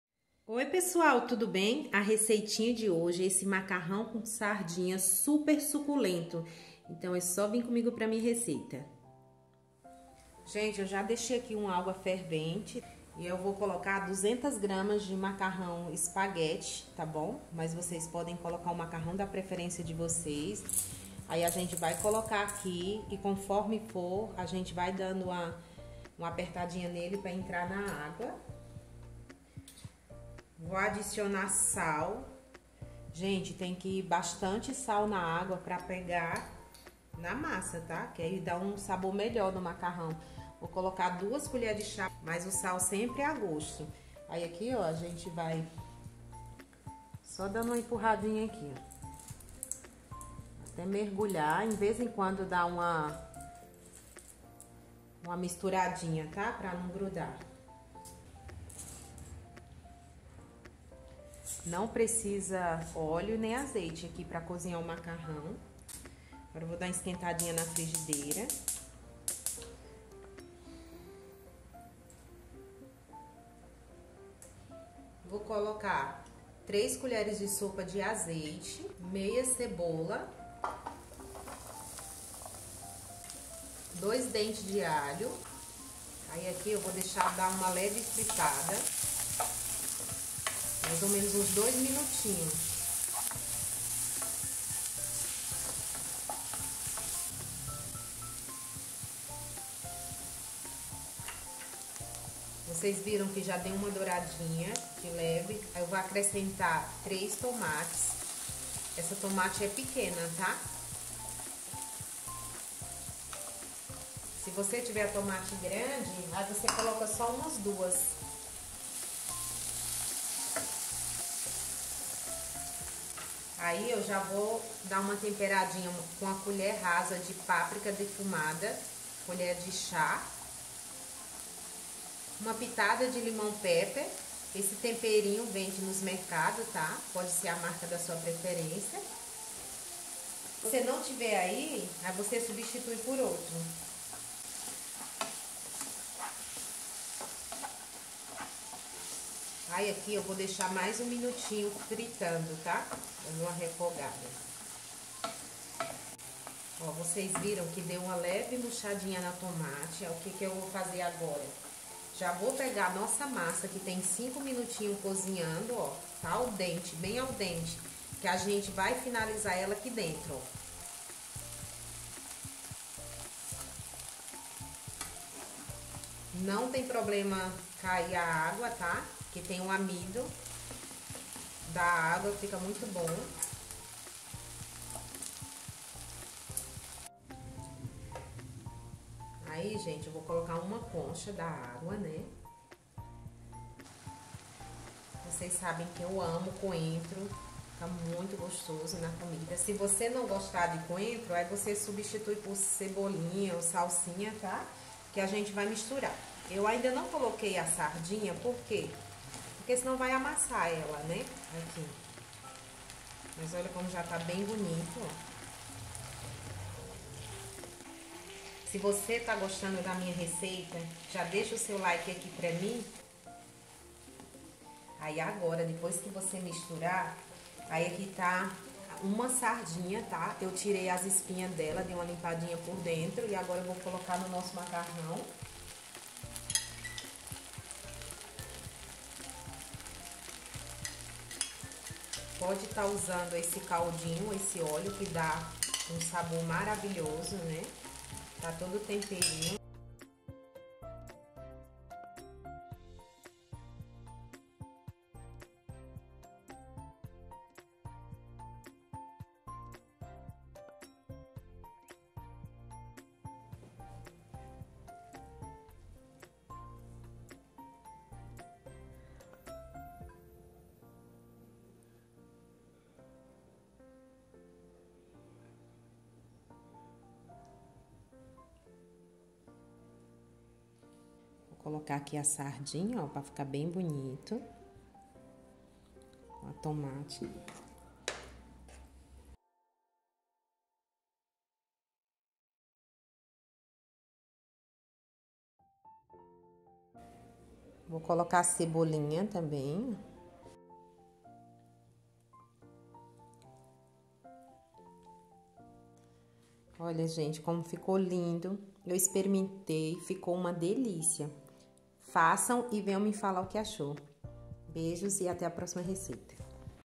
Macarrão com sardinha: saboreie esse novo jeito de inovar no seu macarrão!
Vem aprender como fazer um delicioso macarrão com sardinha para arrasar na cozinha.

Ingredientes:
200g de macarrão.
Sal.
Azeite.
1/2 cebola.
2 dentes de alho.
3 tomates pequenos.
1 colher de chá de páprica.
Lemon Pepper (a gosto).
Coentro (a gosto).
1 lata de sardinha.

Essas dicas estão imperdíveis, aqui em casa esse macarrão fez sucesso!